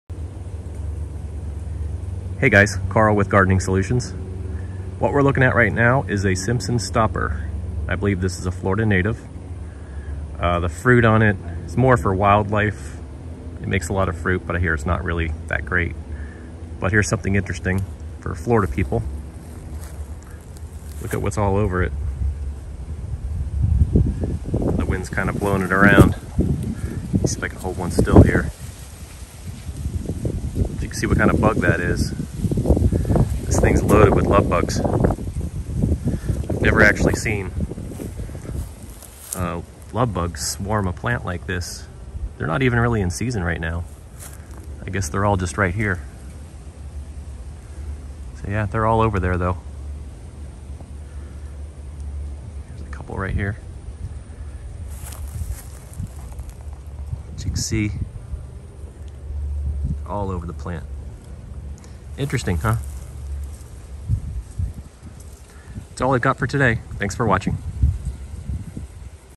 Simpson Stopper the Lovebug Magnet
I took this video about a week ago, lovebugs are now starting to show up in central Florida. This is the first plant I've ever seen them swarm.